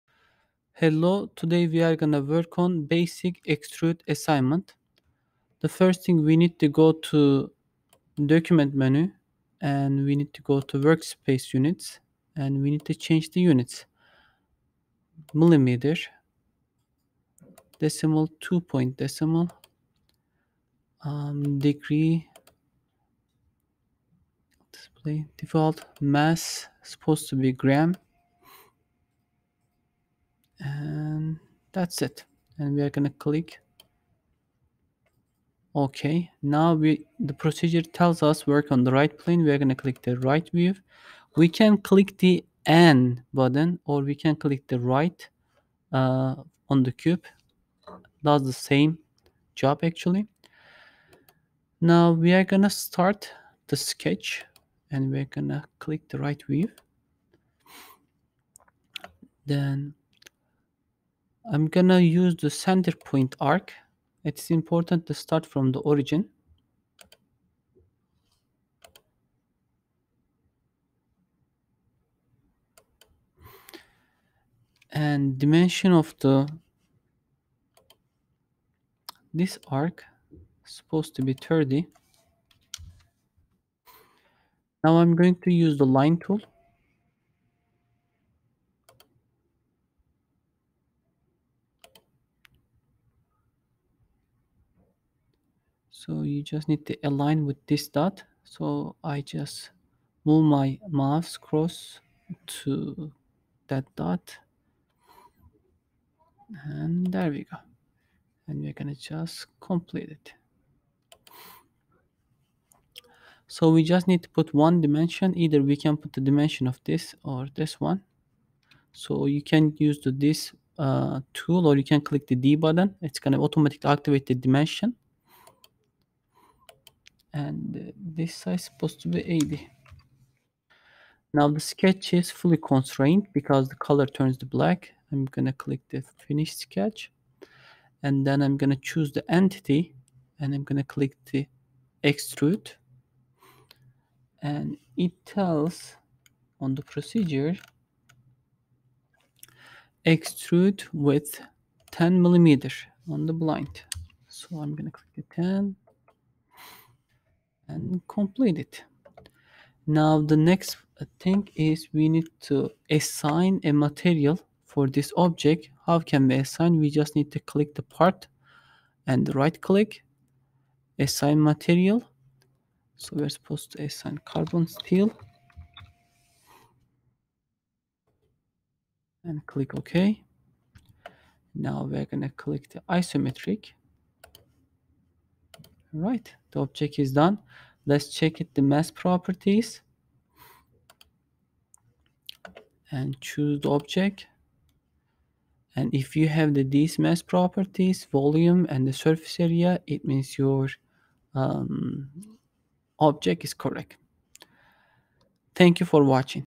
On Shape Practices - Lesson 3 - Basic Extrude
• Create this part from scratch
• Ensure that the Sketch is Fully Defined using
Dimensions and Constraints
• Create an Extrude Feature
• Ensure that the orientation of the part shown
matches your Isometric View